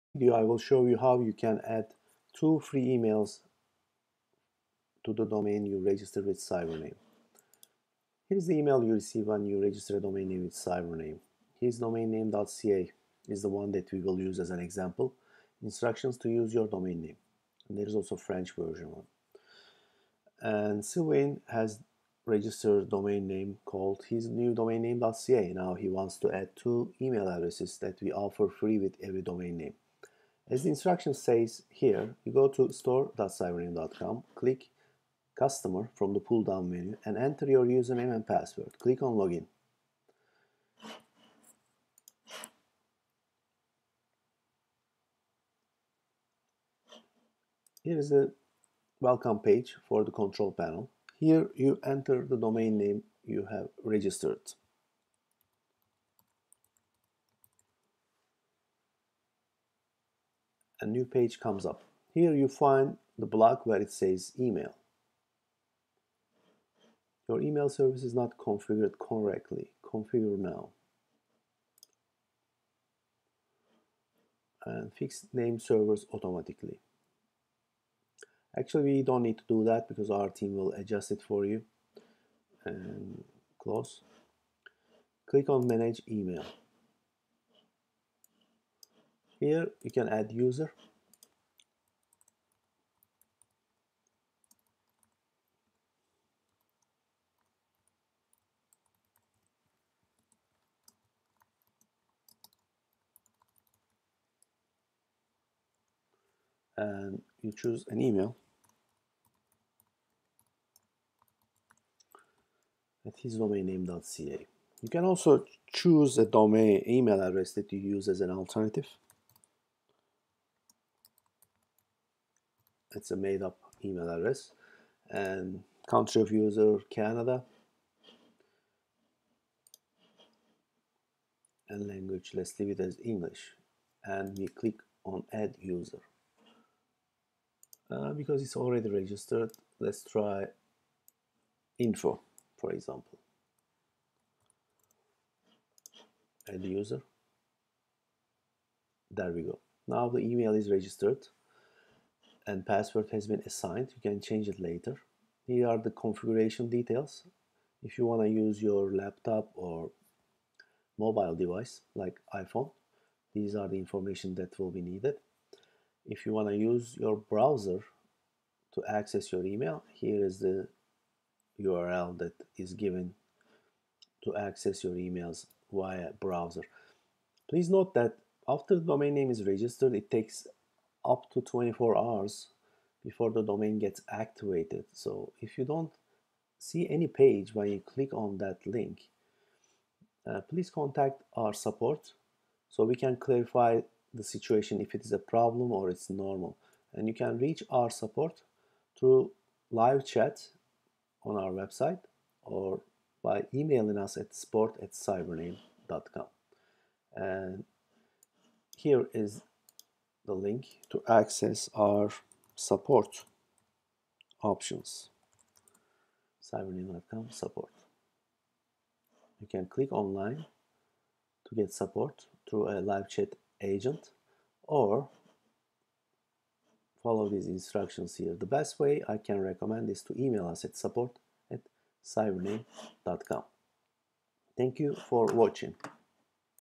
Adding 2 free emails to your domain name
In this short video, we show how to add 2 free emails to your domain name,